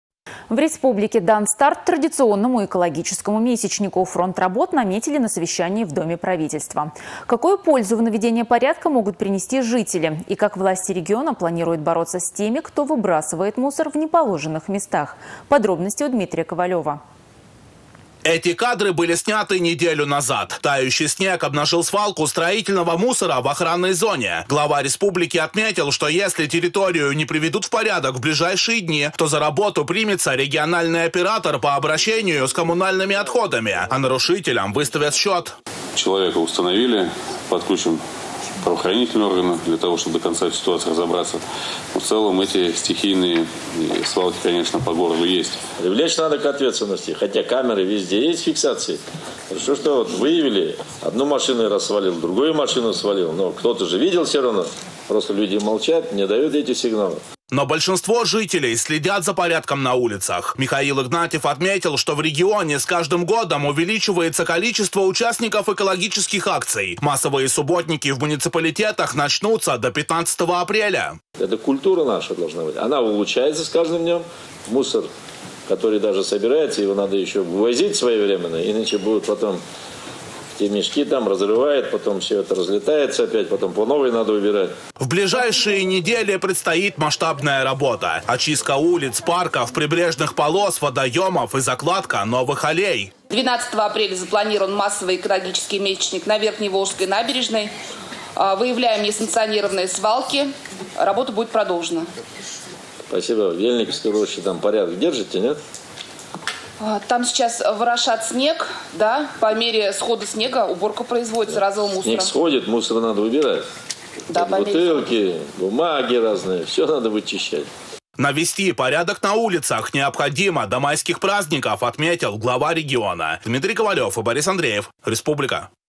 В республике дан старт традиционному экологическому месячнику.
Фронт работ наметили на совещании в Доме правительства. Какую пользу в наведении порядка могут принести жители? И как власти региона планируют бороться с теми, кто  выбрасывает мусор в неположенных местах?